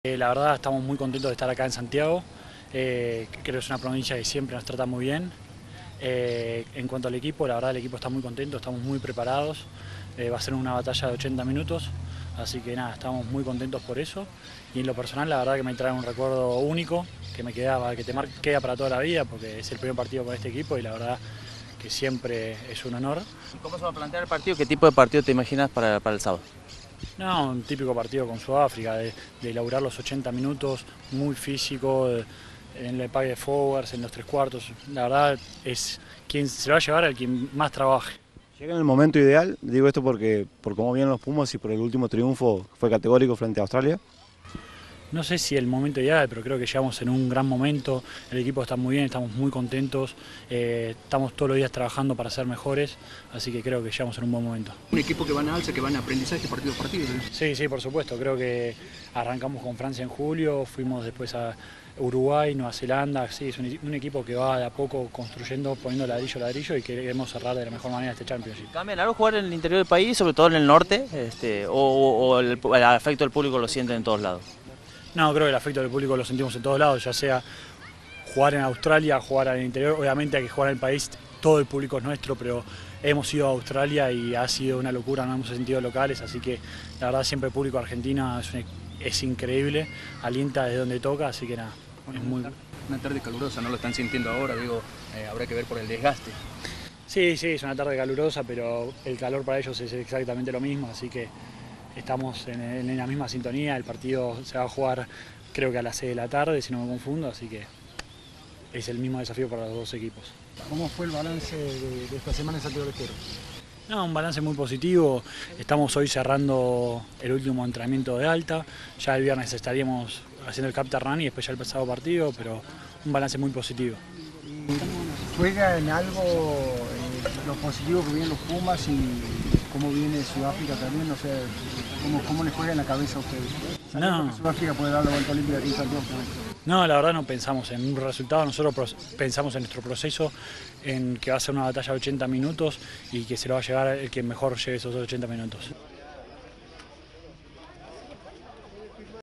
Ignacio Ruiz / foward de Los Pumas
El jugador del seleccionado argentino de rugby palpitó el choque del sábado ante Sudáfrica que se disputará en Santiago del Estero por la quinta fecha del Rugby Championship.